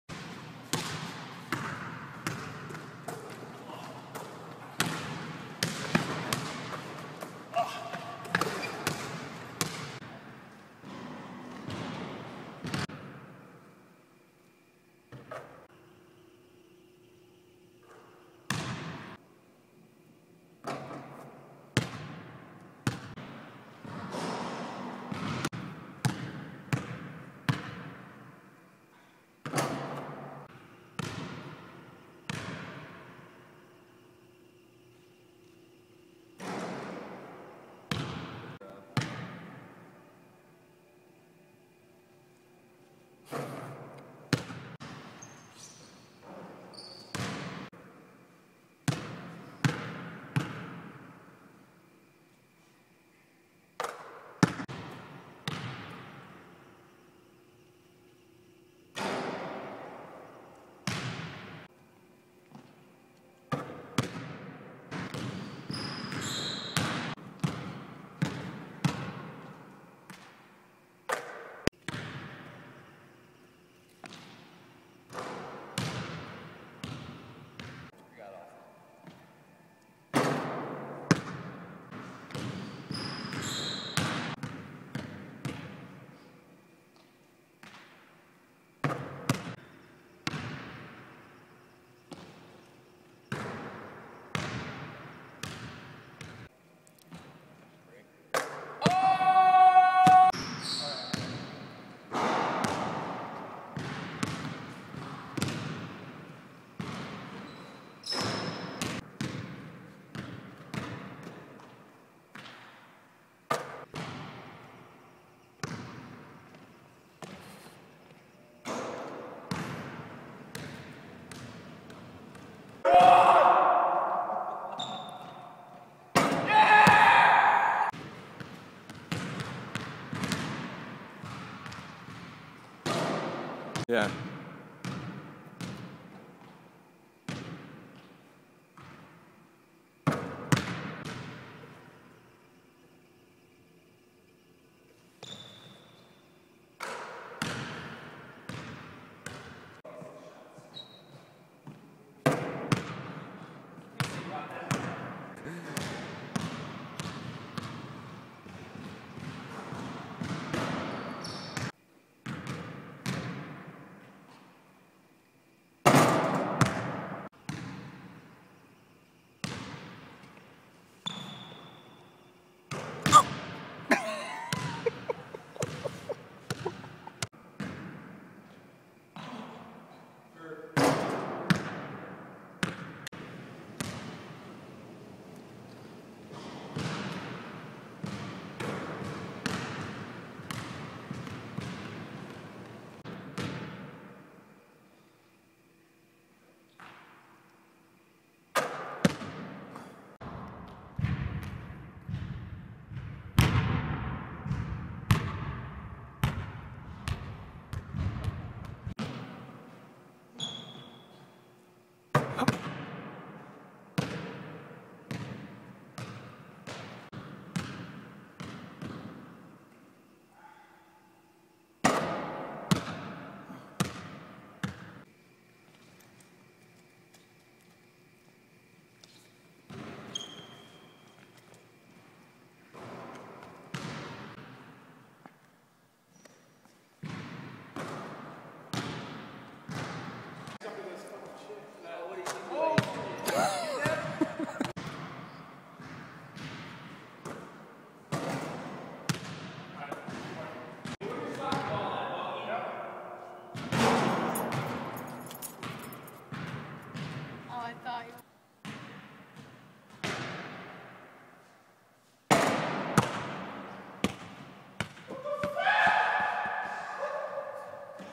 SNAPCHAT - Basketball Shots -
shooting around haha hope you like my vid ^^ i love basketball haha almost as much as i like football (soccer) and team germany ;)